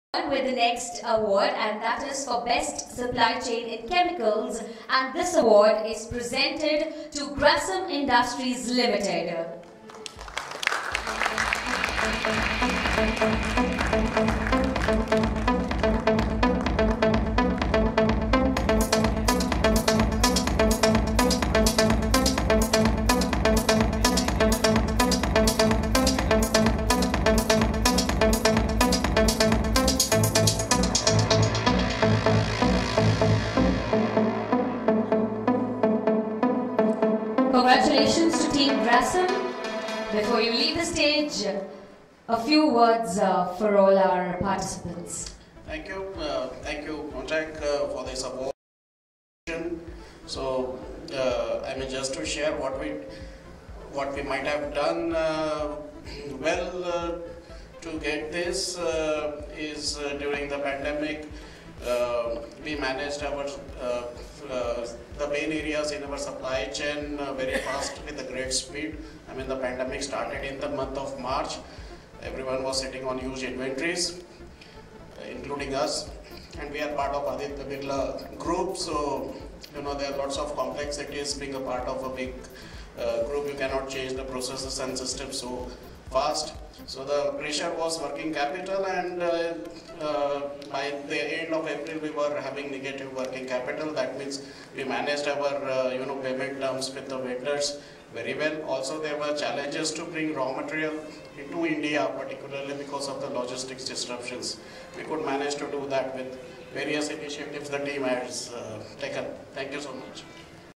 Supply Chain Excellence - Best Supply Chain in Chemicals - Grasim Industries Ltd
The recently conducted Warehouse & Logistics Excellence Awards 2021 seen Grasim Industries Ltd bag the award for the Supply Chain Excellence - Best Supply Chain in Chemicals for the Year 2021